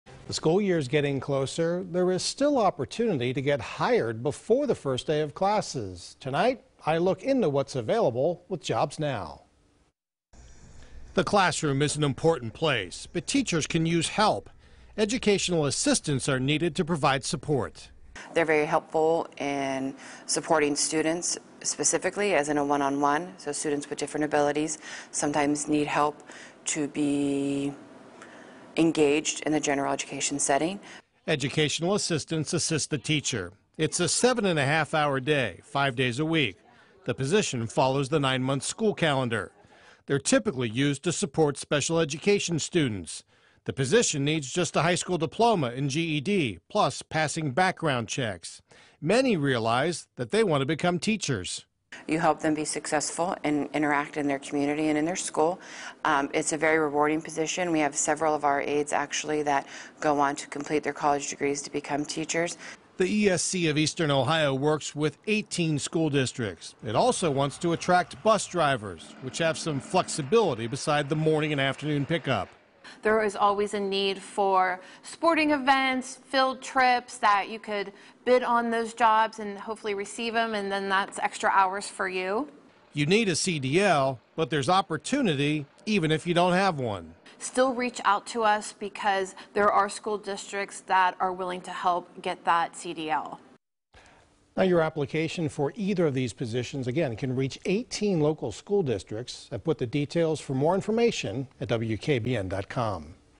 JobsNOW: Local schools looking for educational assistants, bus drivers
The ESC of Eastern Ohio works with 18 school districts. It also wants to attract bus drivers, which have some flexibility besides the morning and afternoon pickup.